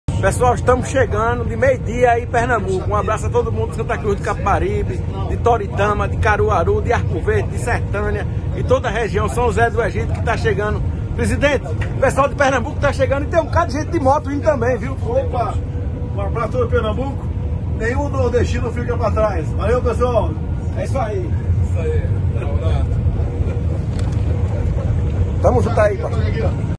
Bolsonaro: Nenhum nordestino vai ficar para trás; assista
Em um vídeo grava agora há pouco a bordo do avião presidencial vindo para Pernambuco inaugurar a Adutora do Agreste que vai levar água do Rio São Francisco para 2 milhões de pessoas, o ministro do Turismo Gilson Neto, pegou uma rápida fala do presidente Jair Bolsonaro, dizendo que nenhum nordestino vai ficar para trás no seu governo. Assista: